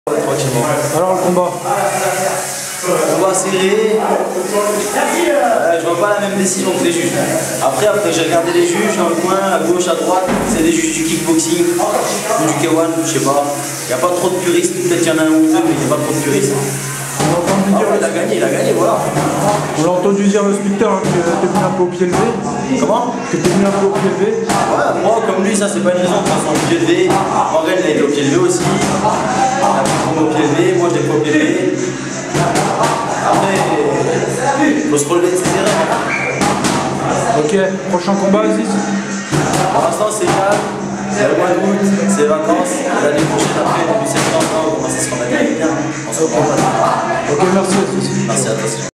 ITV Aziz HLALI Best Of Siam 5
14 Juin 2014
Défaite aux points face à Morgan ADRAR